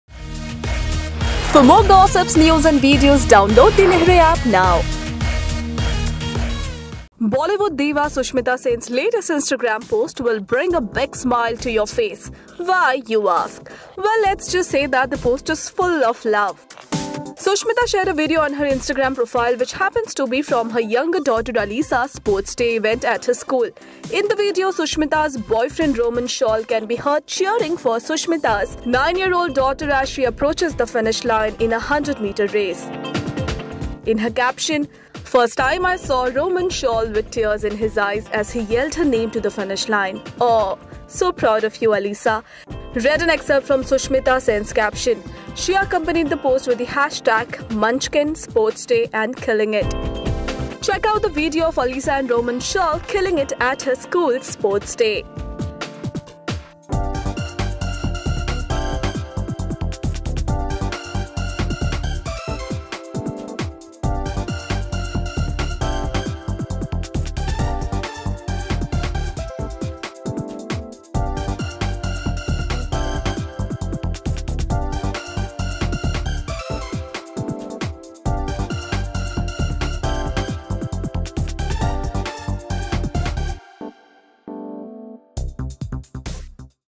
Sushmita Sen's Bf Rohman Shawl Cried When Daughter Alisah Wins The Race | LehrenTV
On Friday evening, Sushmita shared a video on her Instagram profile, which happens to be from her younger daughter Alisah's sports day event at her school. In her caption, the 43-year-old actress revealed that it was for the first time that she saw Rohman Shawl teary-eyed.